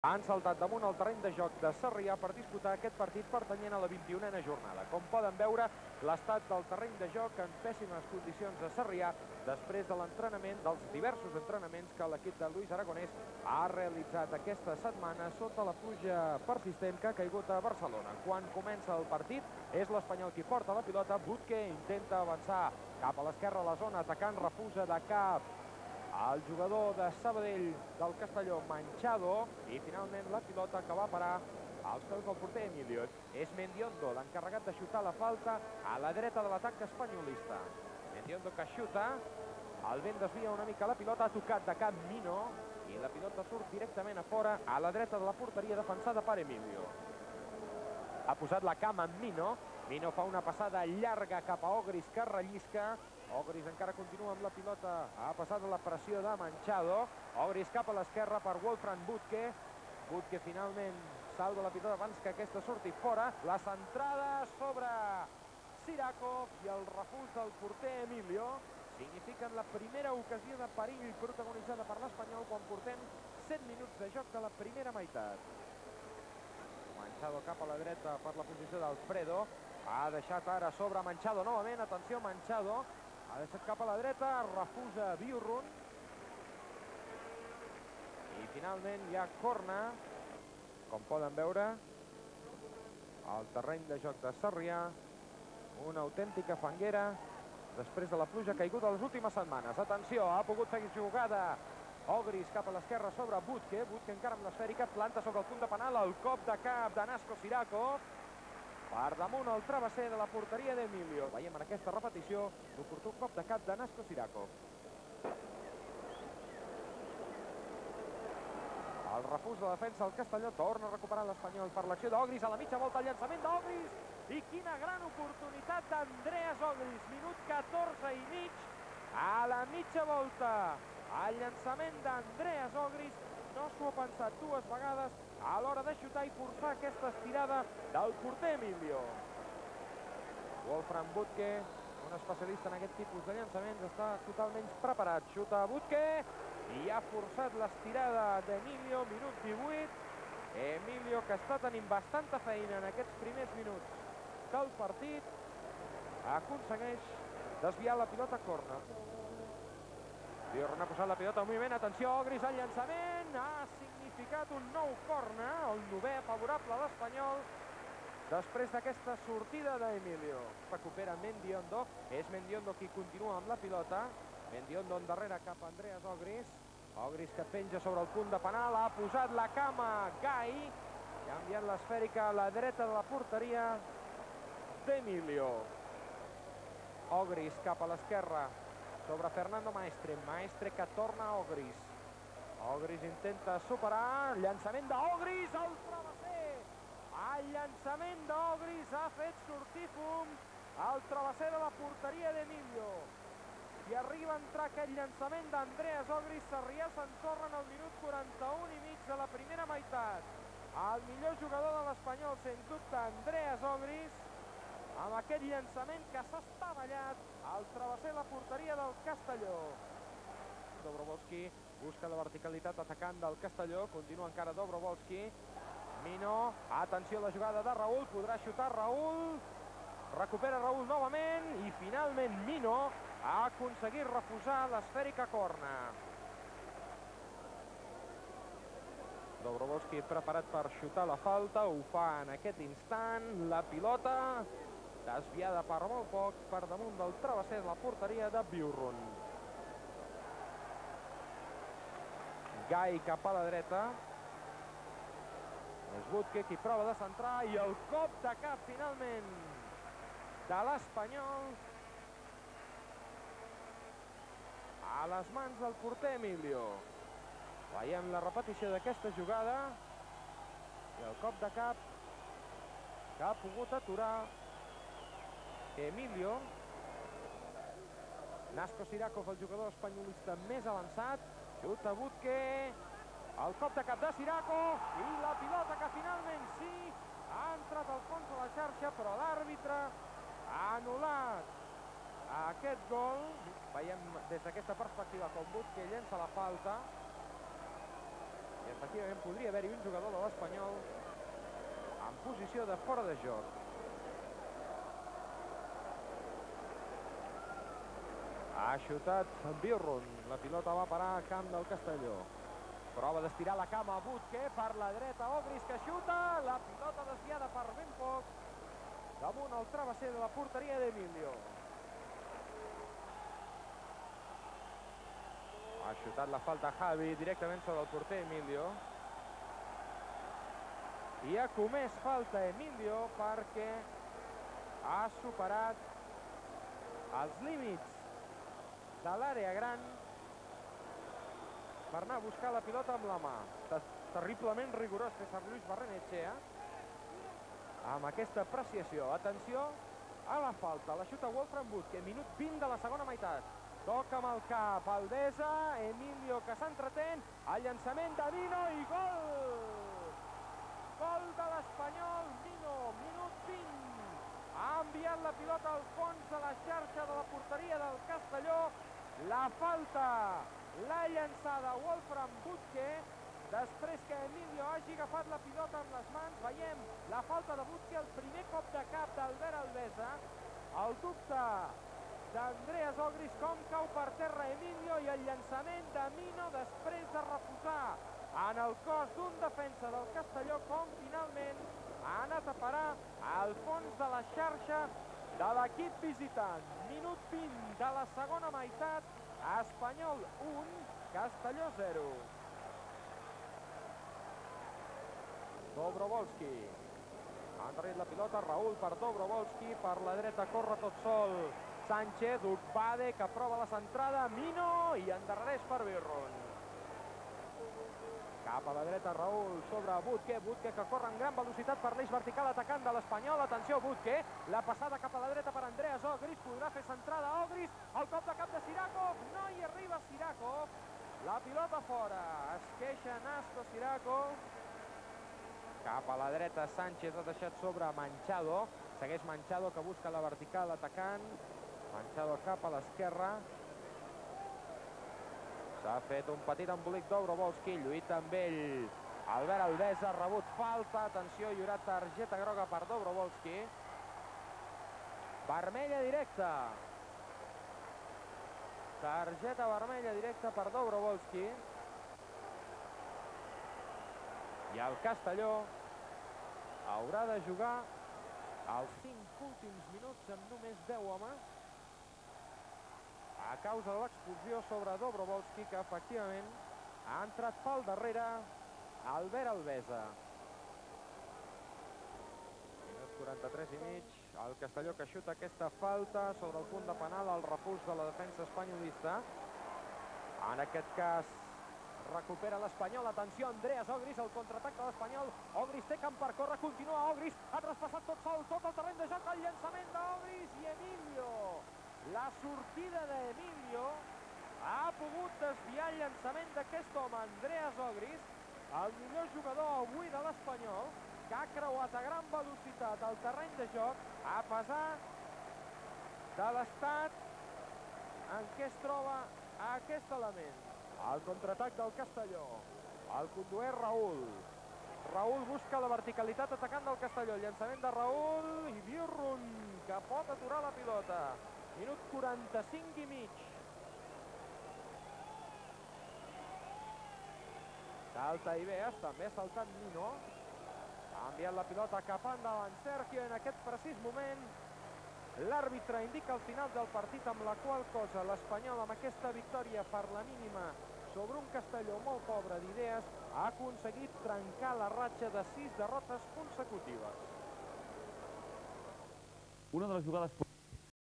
RDC Espanyol - CD Castellón (1-0) / 1ª División - 3 febrero 1991
Un disparo de Mino que rebotó en Ximet, dio la victoria al Espanyol, en un campo muy embarrado, que hizo difícil la práctica del futbol.